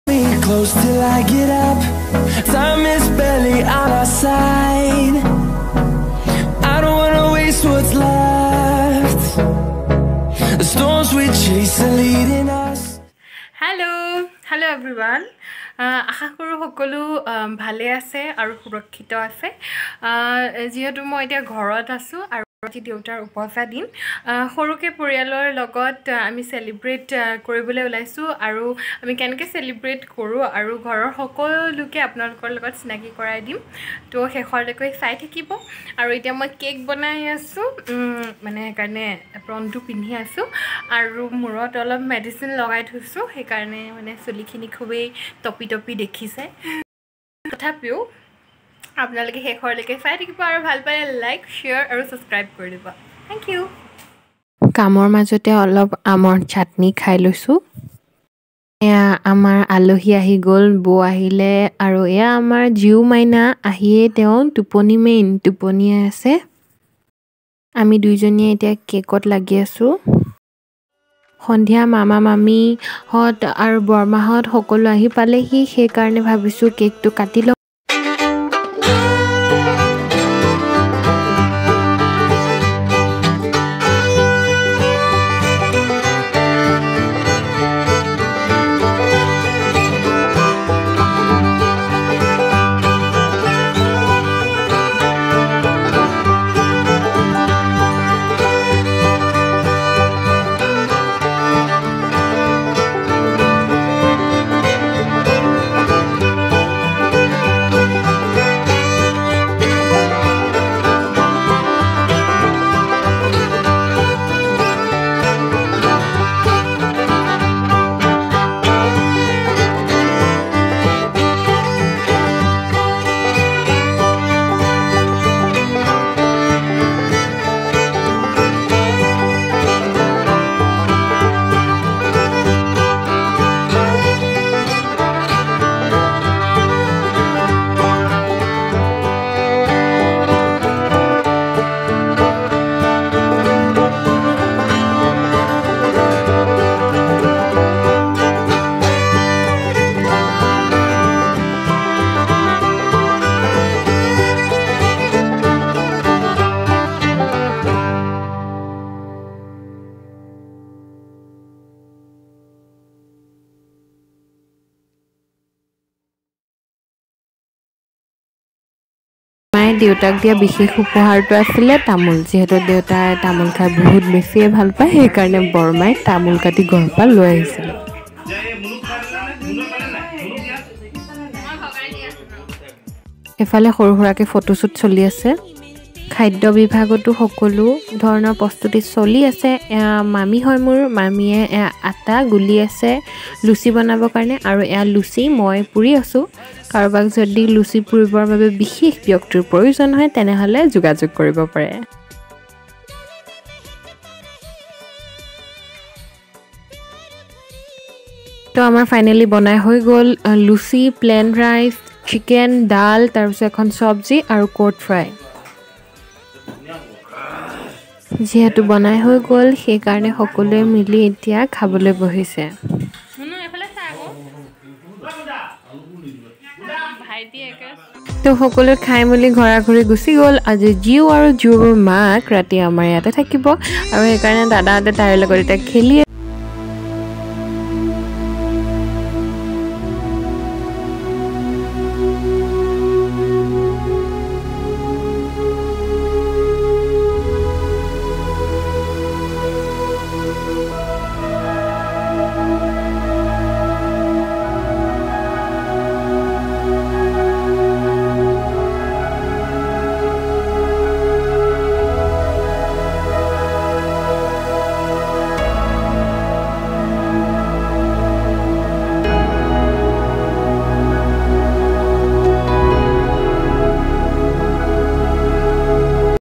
Father's Birthday Celebration || দেউতাক কোনে কি উপহাৰ দিলে || কি কি কৰিলো চাওঁ আহক
hello lovelies I hope all are doing well and safe .